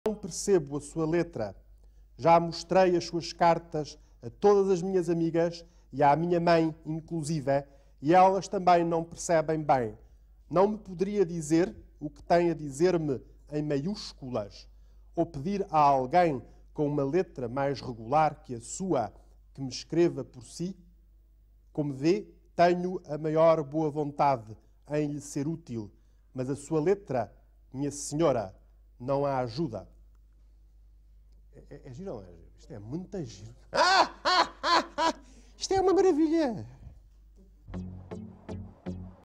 Pedro Paixão lê Adília Lopes
O MARQUÊS DE CHAMILLY A MARIANNA ALCOFORADO (1992)

Minha senhora deve ter
uma coisa muito urgente e capital
a dizer-me
porque me tem escrito muito
e muitas vezes
porém lamento dizer-lho
mas não percebo
a sua letra
já mostrei as suas cartas
a todas as minhas amigas
e à minha mãe
e elas também não percebem bem

não me poderia dizer
o que tem a dizer-me
em maiúsculas?
ou pedir a alguém
com uma letra mais regular
que a sua
que me escreva
por si?
como vê tenho a maior boa vontade
em lhe ser útil
mas a sua letra minha senhora
não a ajuda